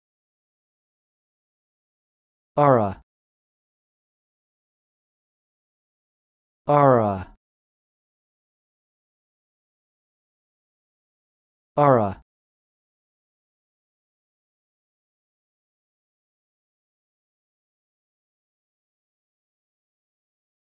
Learn how to say this word: "Ara"
This is the definition of "Ara":
n.  The Altar; a southern constellation, south of the tail of the
   Scorpion.